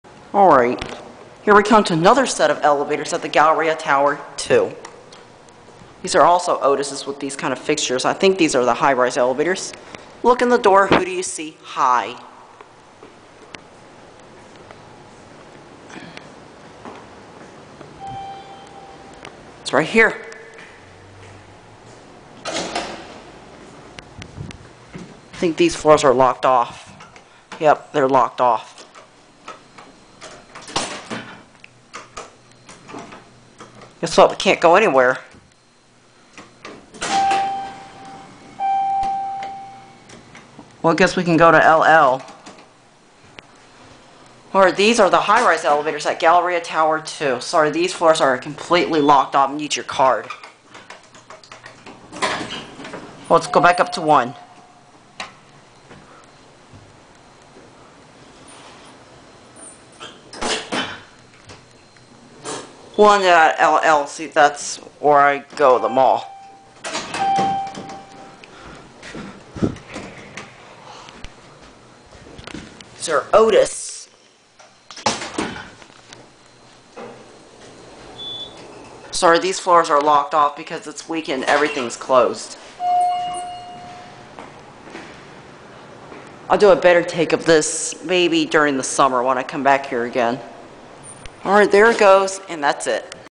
An EPIC FAIL on the Otis High-Rise Traction Elevators at Galleria Tower II in Houston, TX.
All the floors are restricted, because it is Saturday. Only it works is LL and 1.